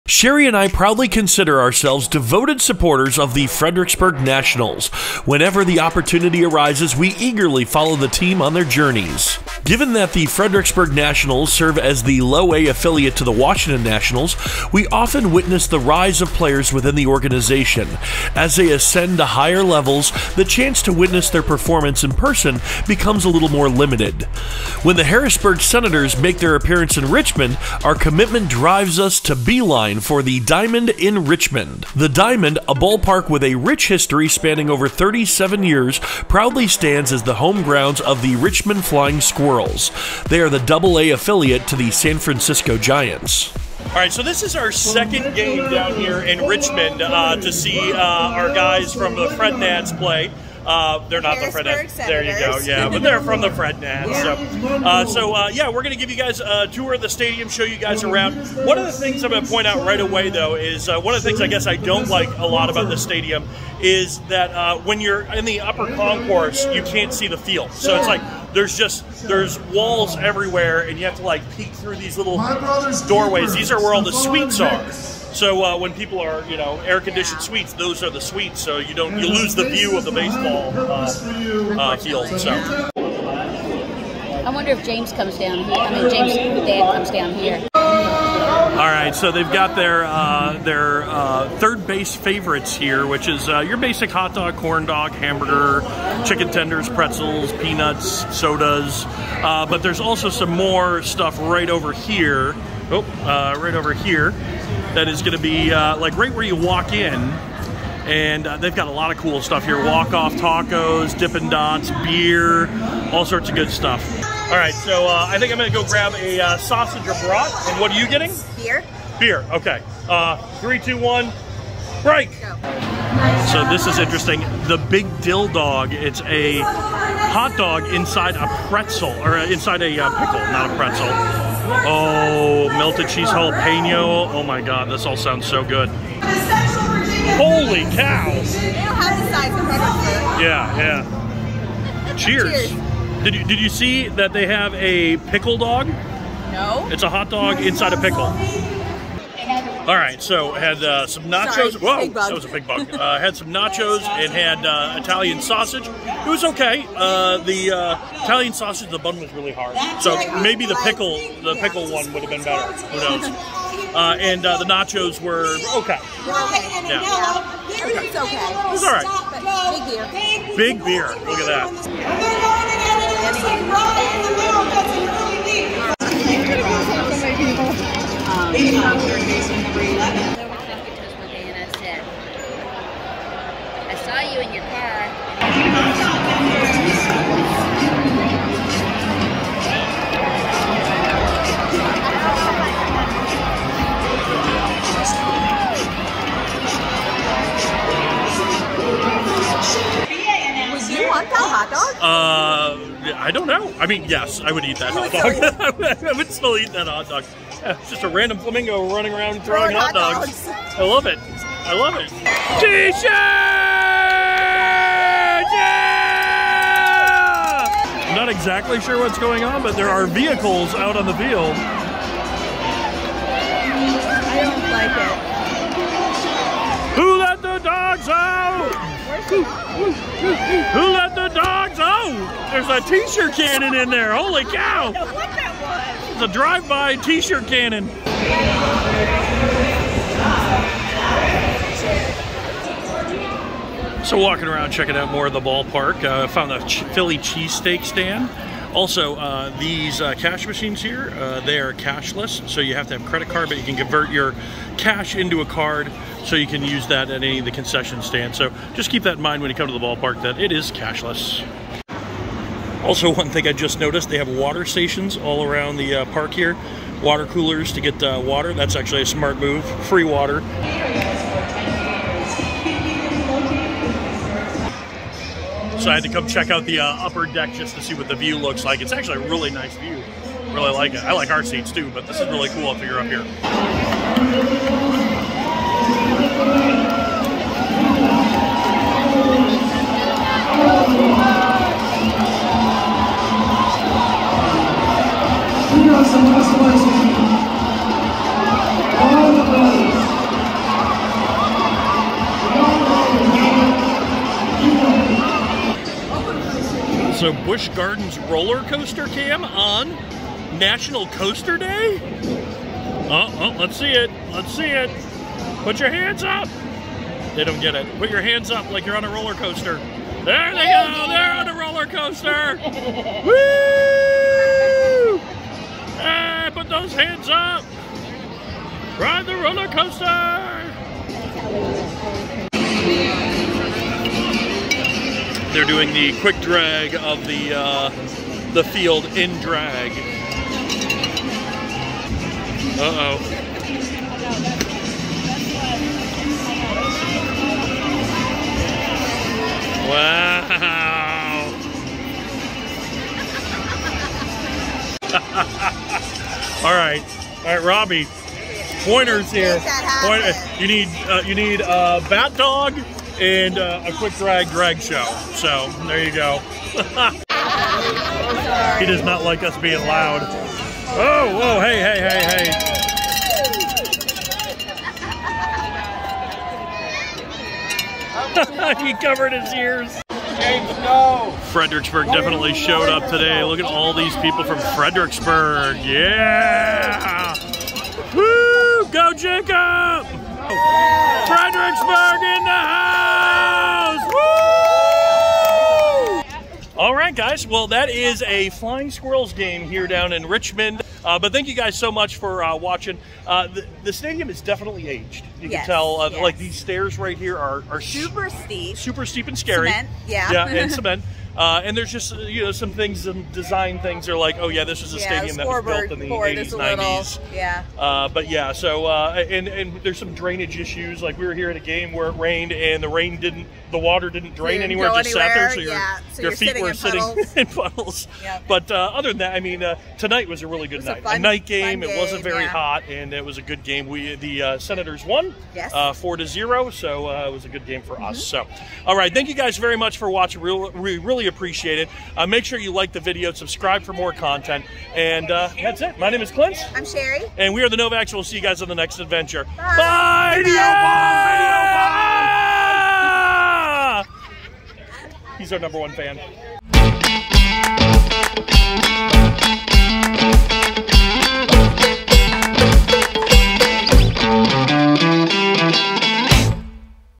Exploring The Diamond: A Tour of Richmond's Historic Baseball Stadium
Join us on an immersive journey through the iconic baseball haven of Richmond, Virginia – The Diamond! In this video tour, we take you behind the scenes of this 37-year-old sporting gem that has witnessed countless thrilling moments and holds a special place in the hearts of sports enthusiasts. From the historic grandstands to the vibrant atmosphere, you'll get an exclusive glimpse of what makes The Diamond an enduring symbol of Richmond's sporting spirit. Whether you're a die-hard baseball fan or simply someone who appreciates the essence of historic venues, this tour is a must-watch.